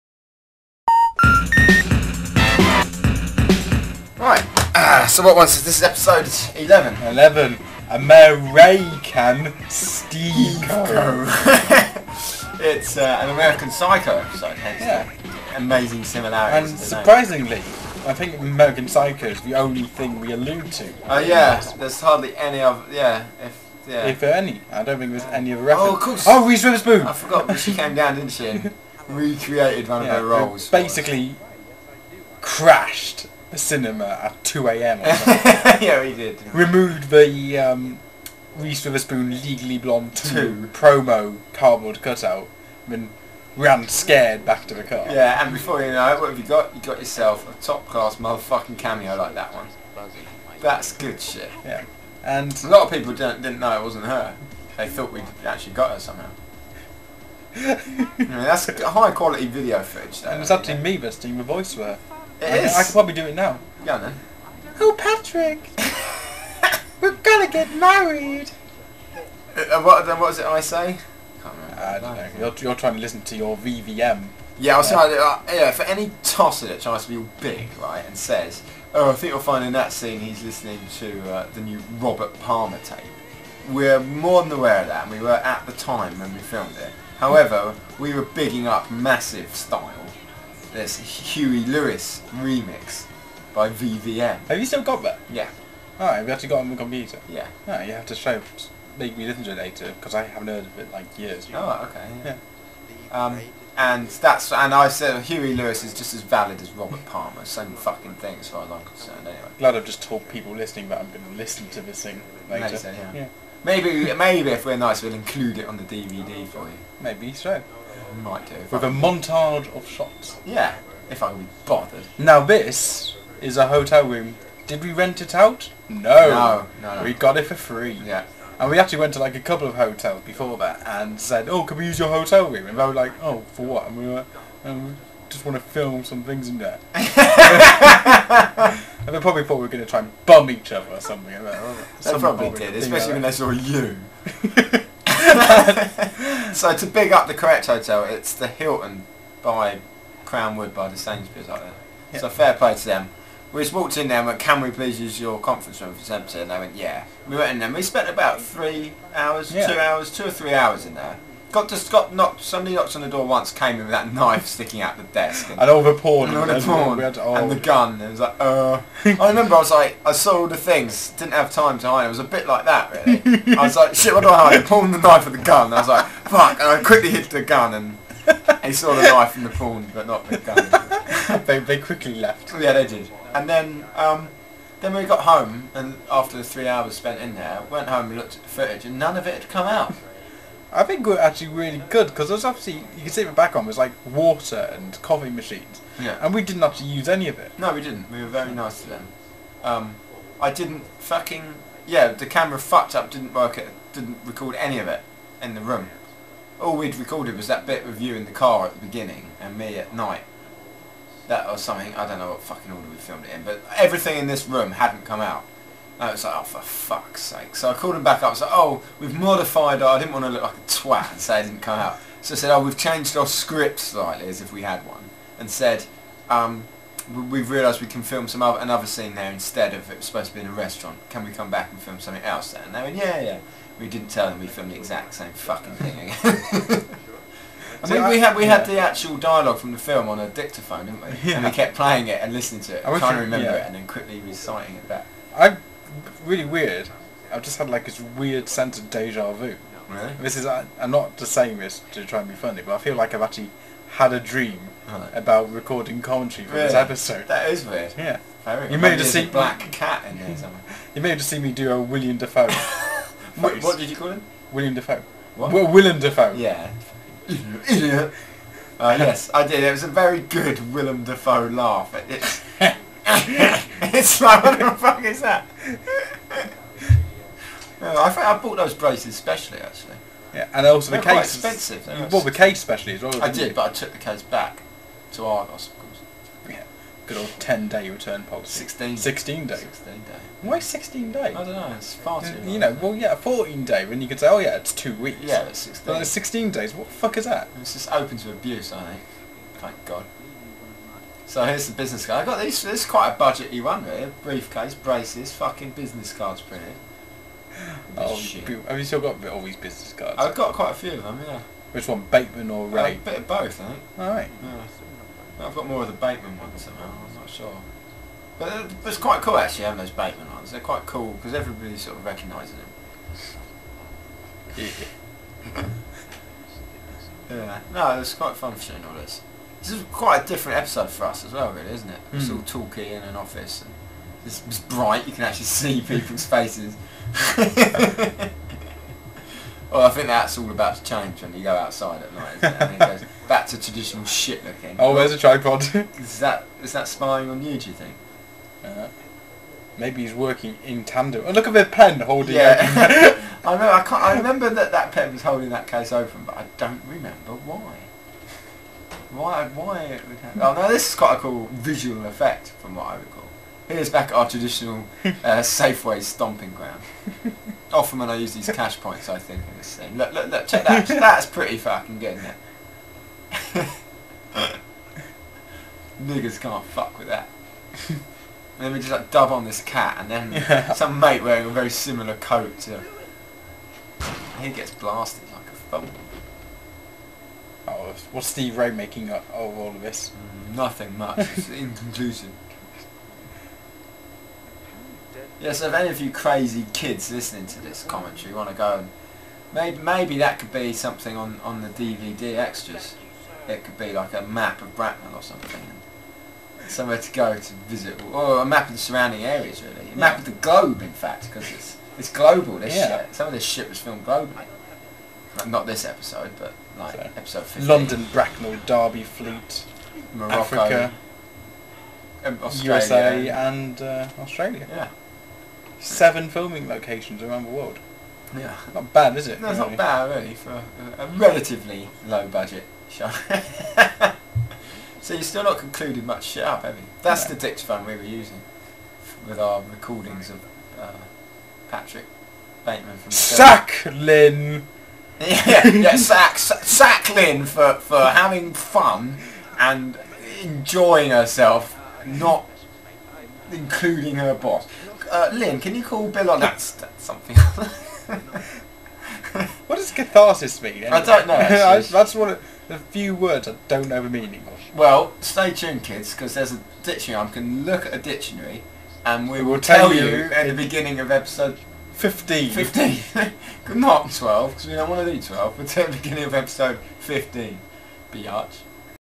SR 11 with Commentary
Steve Ray episode 11 with full commentary by Gaz and Nad. The commentary was recorded around 2006.